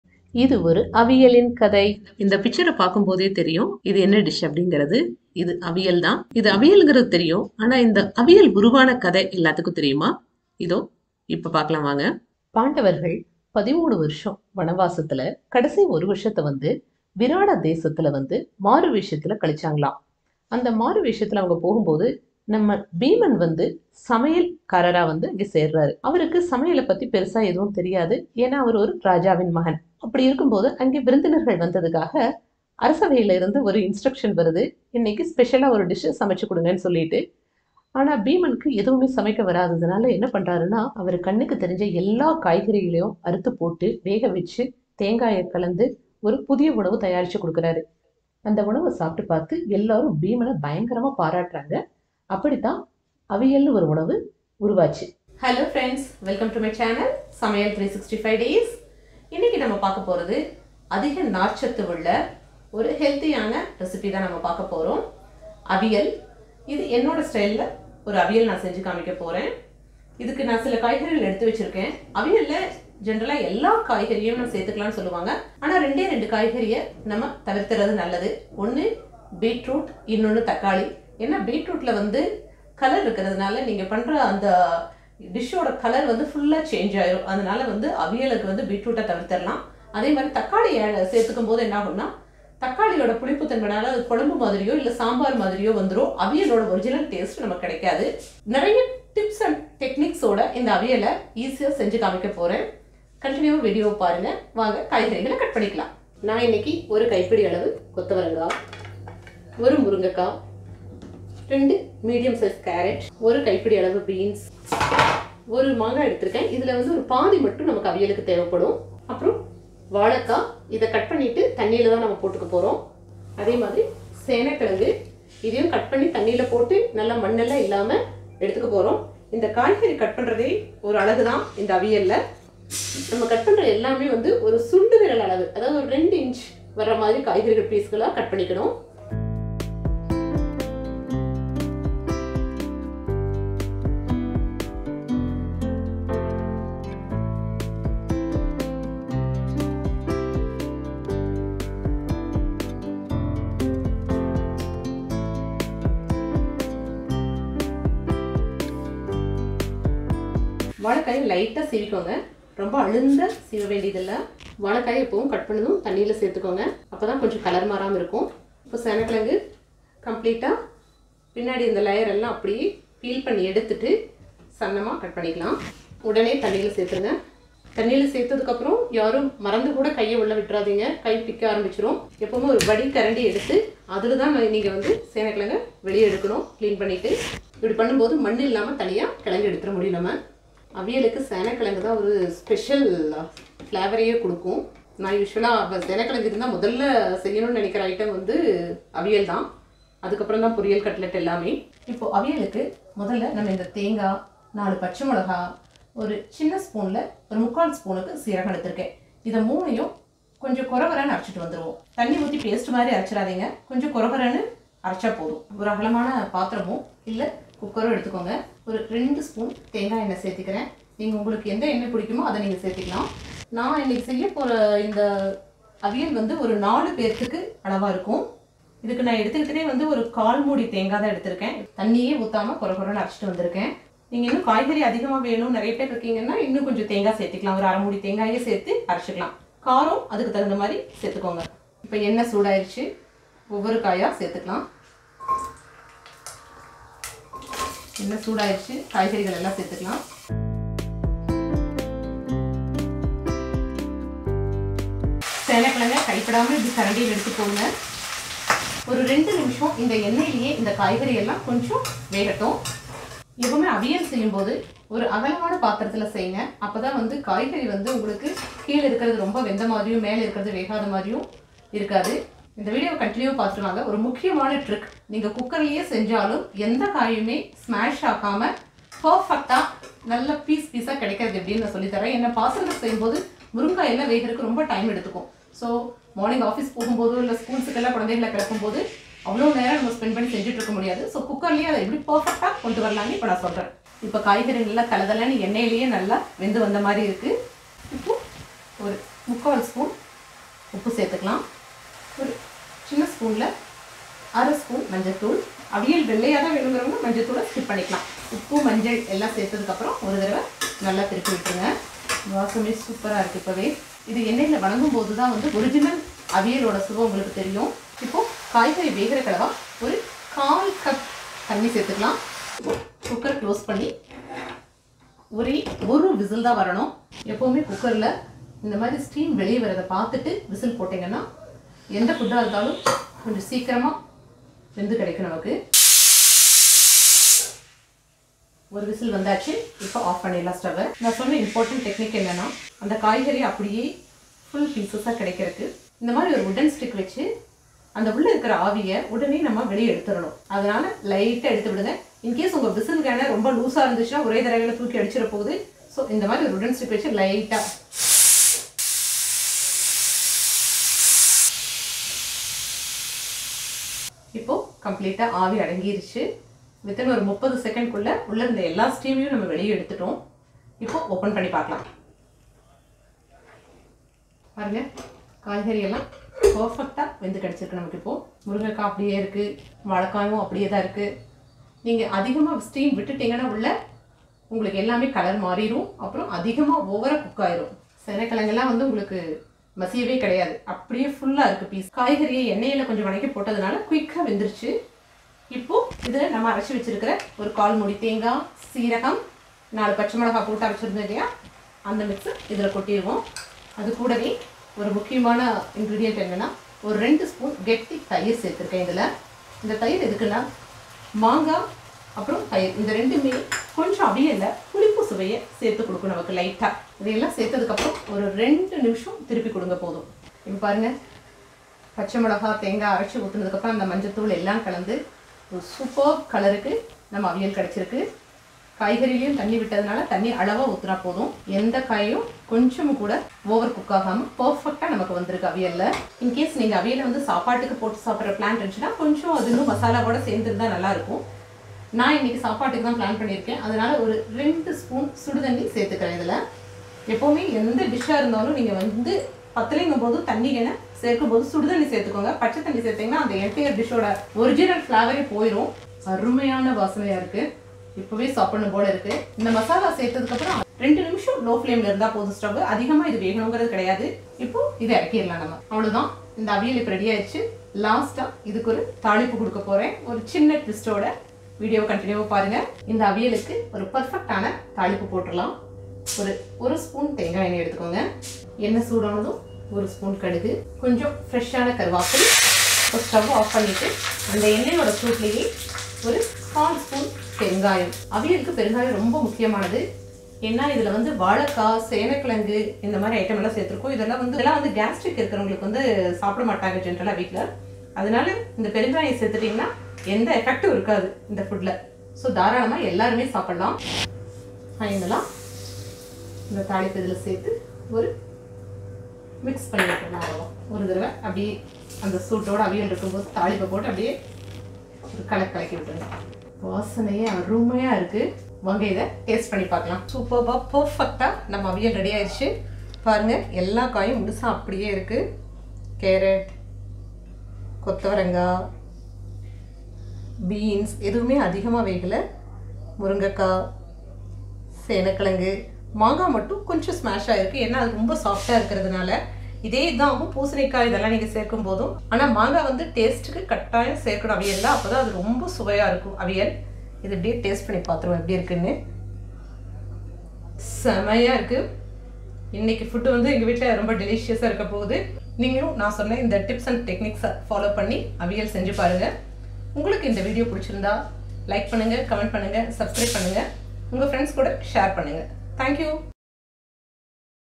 Easy Avial Recipe/Avial Story|அவியல் செய்முறை(English Subtitles)|How to make Aviyal/கேரளா அவியல்
Easy aviyal recipe

Here i have given the story how the dish Aviyal came in to existence.
When pandavas was in exile for 13 years, they spend the the last 1 year in the kingdom of Viraad. There bhima joined as cook in the palace of Viraad king.
He was asked to make a special dish for the guest but bhima didn;t know to cook, so he cut all the vegetables he has seen and cooked in water and added coconut and made a new dish. which everyone liked and appreciated bhima. this is how aviyal came in to existence.

Add 2 spoons of oil, add all veggies (drumstick, carrot, beans, cluster beans, plantain, raw mango, yam) saoute the veggies in oil for 2 minutes, add 1 spoon salt and 1/2 spoon turmeric powder and allow it cook for 1 whistle.

as soon the whistle came just remove the steam slowly.

grind  coarsely 1/4 cup coconut with 1 spoon cumin seeds and 4 green chillies. without adding water.

Add the grinded paste with the veggies and give a mix. add 2 spoons of thick curd aswell and cook in low flame for 2 minutes. if needed add little hot water to adjust the consistency.

finally put the tempering adding 1 spoons of oil add mustard seeds, little curry leaf and Hing (asafoetida)
Add the tempering in the veggies and mix slowly.

Now Aviyal is Ready to serve and eat.

lunch menu
healthy food tips
samayal 365 days